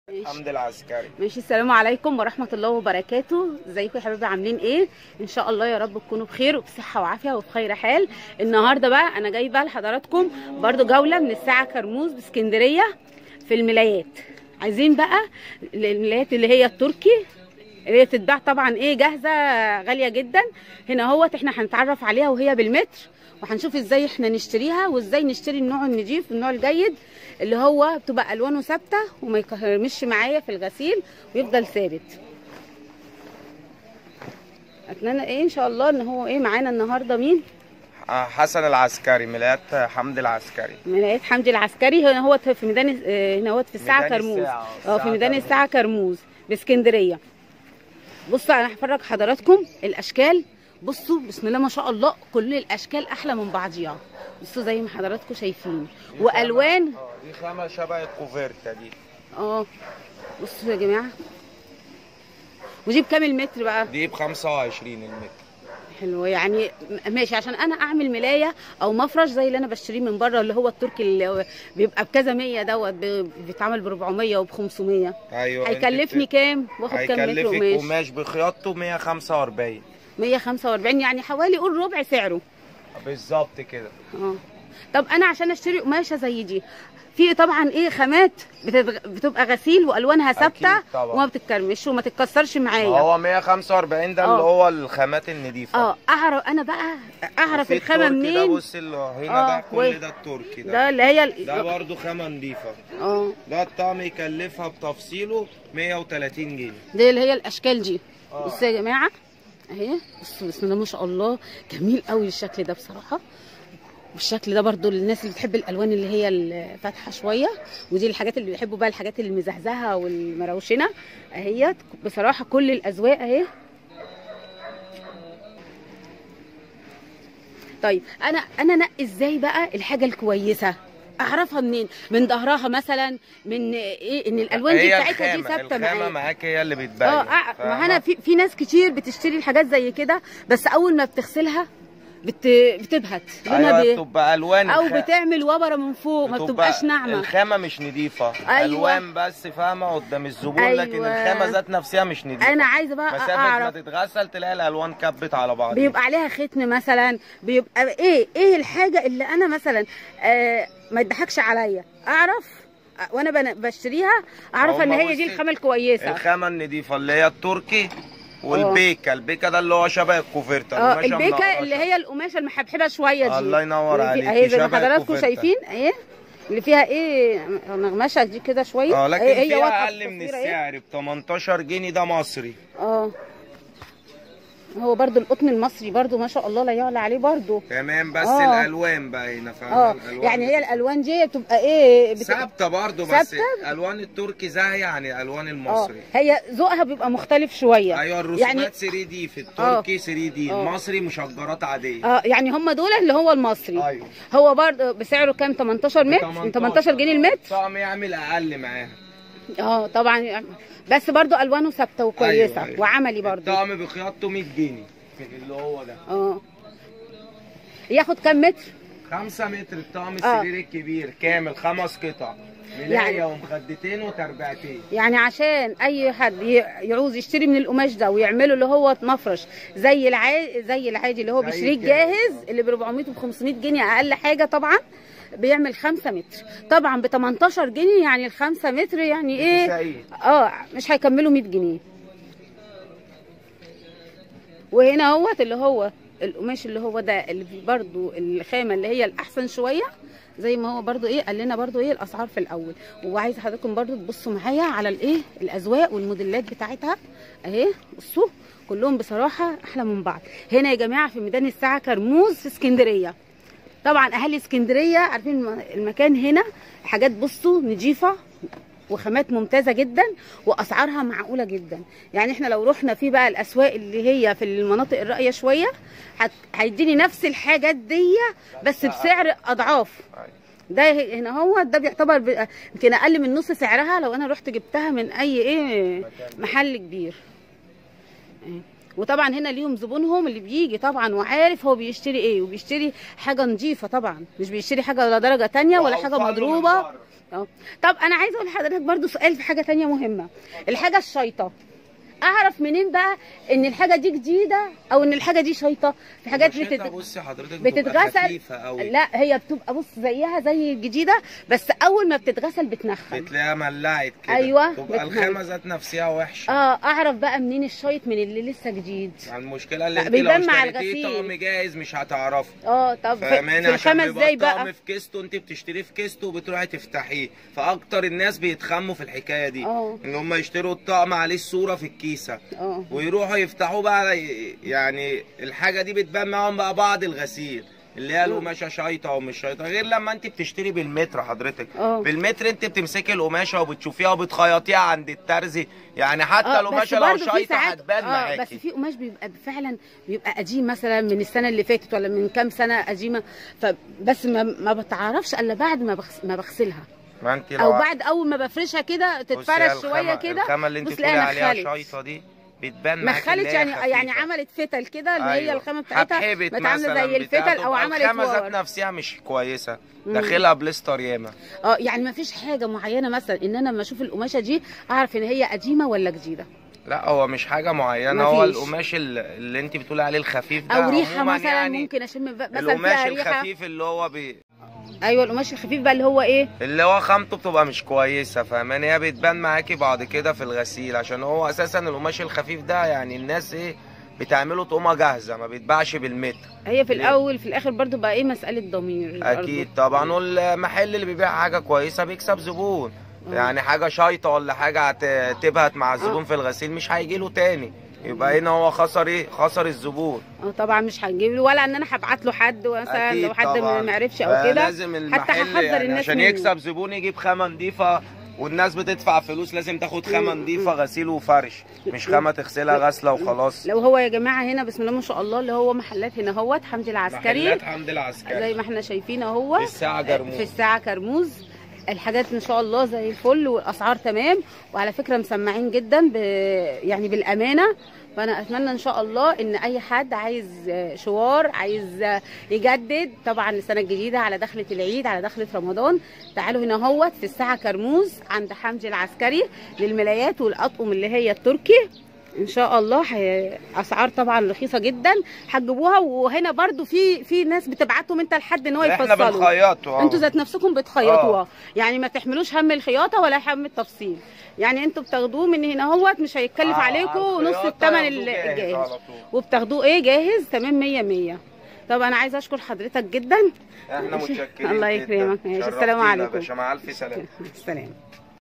جوله فى الساعه كرموز اسعار قماش الملايات بارخص الاسعار
ملايات كرموز ملايات سرير ارخص الاسعار لقماش الملايات#اسعارقماش الملايات